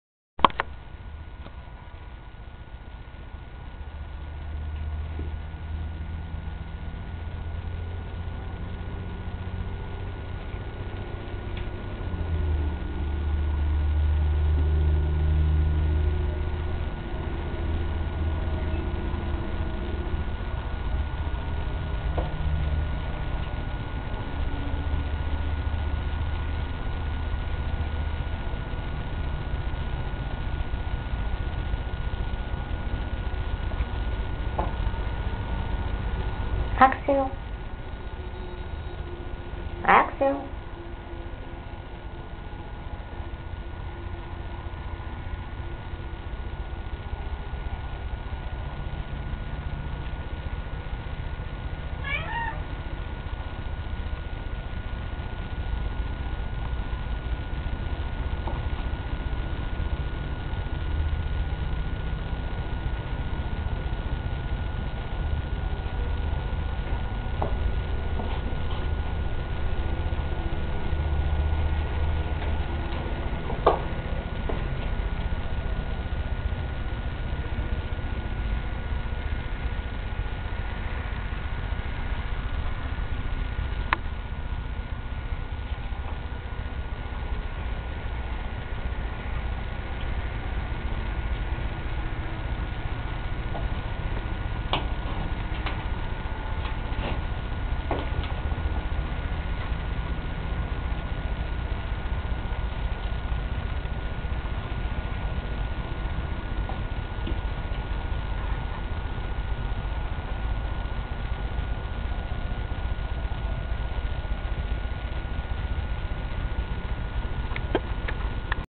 AXEL AND MONIKA
MY CAT AXEL AND MY POODLE MONIKA LIKE SUN